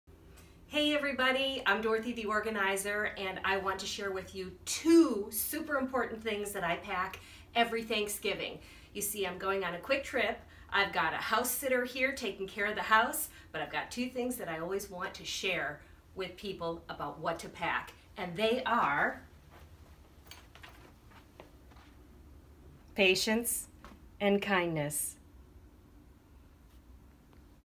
Dorothy the Organizer recommends that you take these two things with you to Thanksgiving.
Dorothy the Organizer, Expert on Hoarders is always looking for ways to help her community. Check out this 28 second video to learn the top 2 Most important items to take with you to Thanksgiving.